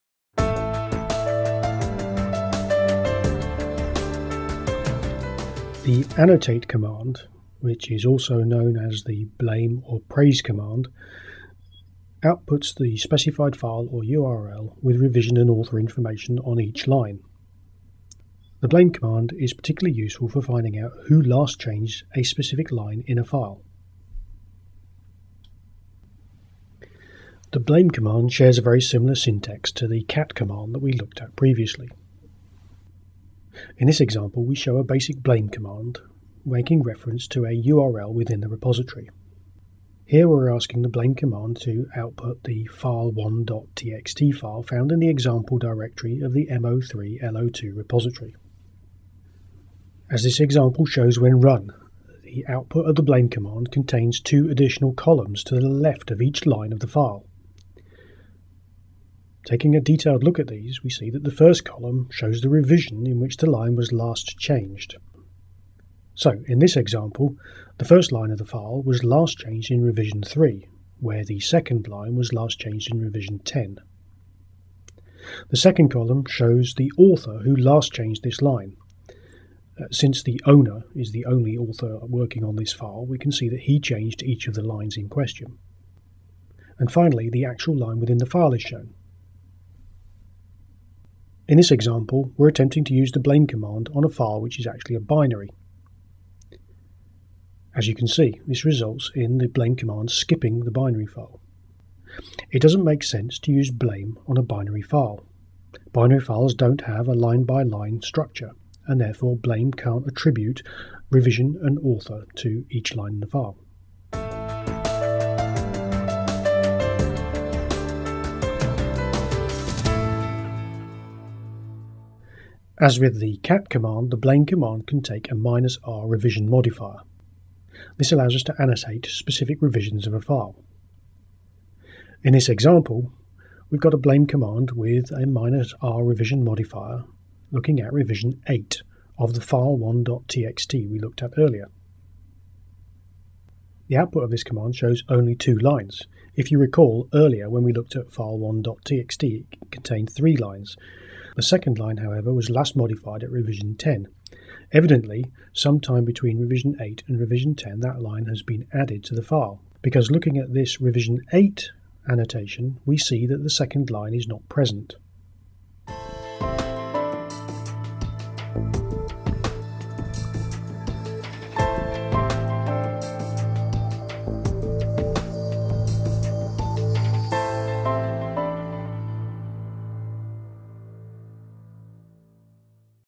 115_03-Subversion: blame; browsing the repository
Browsing with the svn annotate/blame/praise commands

*Best watched full screen*

This video is a part of the Principia Subversion course available in full